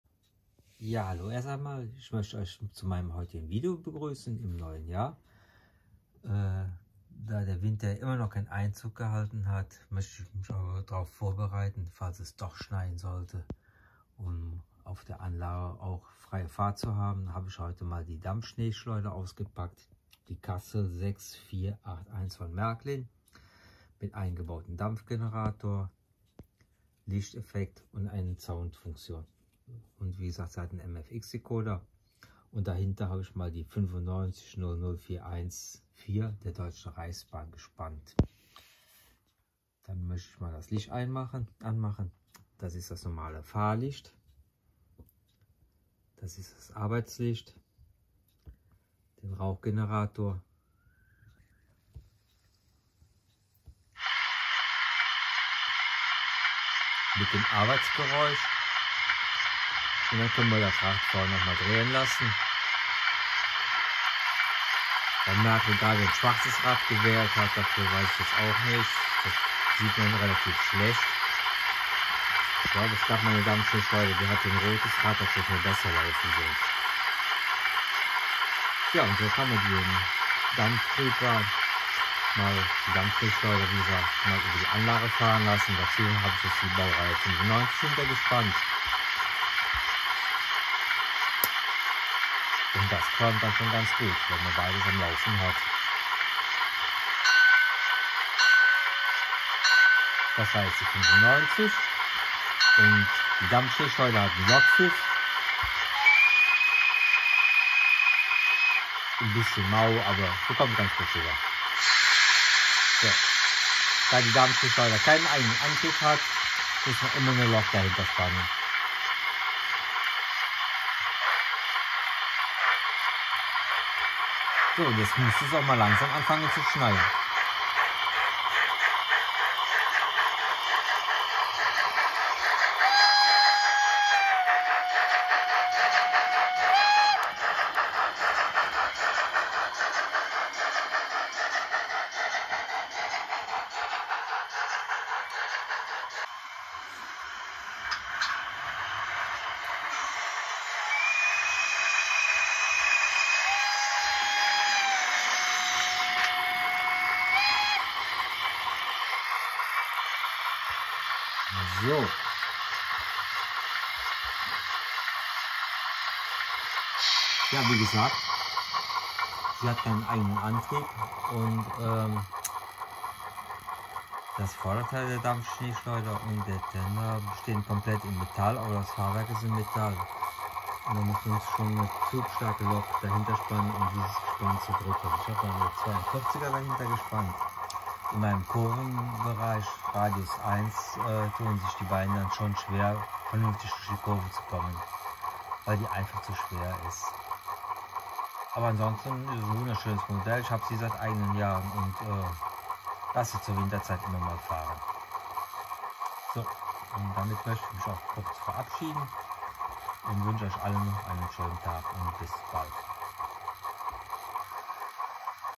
Der Winter kann kommen! Märklin Schneeschleuder
Hallo zusammen, hier ist die Videobeschreibung. Gerne Aufklappen!

Modelle in diesem Video:

Märklin 49955: Dampfschneeschleuder
Märklin 39097: Br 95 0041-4